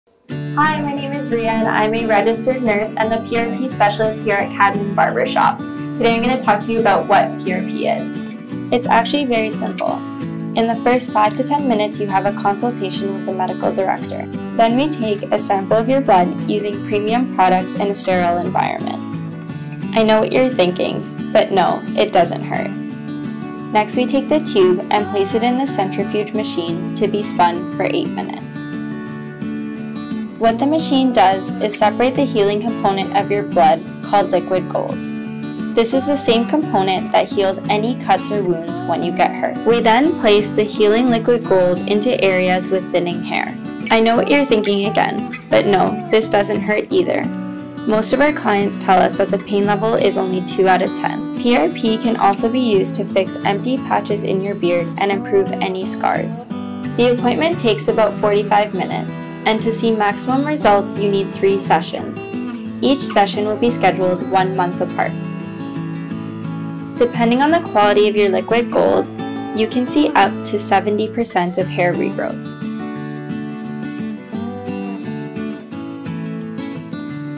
Best Hair Loss Solution? - PRP (Platelet-rich plasma)
This is an educational video about what PRP (Platelet-rich plasma) is and how it works. CADMEN Barbershop provides one of the best hair loss solutions for men.